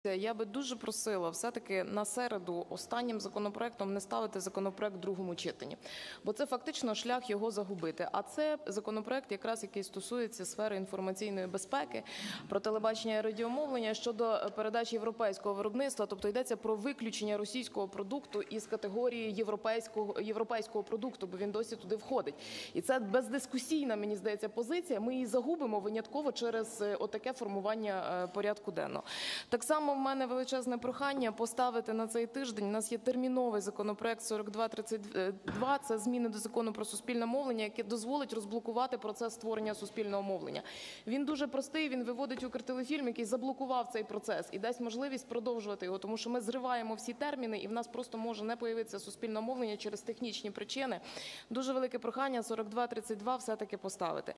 Сюмар закликала ухвалити законопроект, який дозволить розблокувати створення суспільного мовлення
Голова Комітету ВР з питань свободи слова та інформаційної політики, заступник керівника фракції «Народний фронт» Вікторія Сюмар звернулася з проханням до керівництва парламенту внести до порядку денного поточного сесійного тижня терміновий законопроект № 4232, який дозволить розблокувати процес створення суспільного мовлення в Україні.

Про це вона заявила у понеділок під час засідання Погоджувальної ради у Верховній Раді.

За її словами, законопроект про внесення змін до Закону України "Про Суспільне телебачення і радіомовлення" № 4232 передбачає зміну порядку приєднання ДП «Українська студія телевізійних фільмів «Укртелефільм» до публічного акціонерного товариства «Національна суспільна телерадіокомпанія України», що в свою чергу забезпечує завершення створення суспільного мовлення та дає можливість Україні виконати свої міжнародні зобов’язання.

«Ухвалення цього законопроекту дозволить розблокувати процес створення суспільного мовлення. Він дуже простий. Він виводить «Укртелефільм», який заблокував цей процес, і дасть можливість продовжити його. Тому що ми зриваємо всі терміни. І у нас може просто не з`явитися суспільне мовлення через технічні причини. Тому дуже велике прохання законопроект № 4232 все ж таки поставити», - наголосила вона.

Крім того, Вікторія Сюмар заявила про необхідність перенести розгляд законопроекту про внесення змін до закону «Про телебачення і радіомовлення» на початок сесійного дня.

За словами Голови профільного комітету, те, що розгляд цього законопроекту у другому читанні запланований у кінці сесійного дня у середу - "це шлях його загубити".

"А це законопроект про телебачення і радіомовлення, який стосується питання інформаційної безпеки. Йдеться про виключення російського медіа продукту з категорії європейського продукту, бо він досі туди входить. І це бездискусійна, мені здається, позиція. І ми її загубимо через таке формування порядку денного", - заявила вона.

Приєднуйся!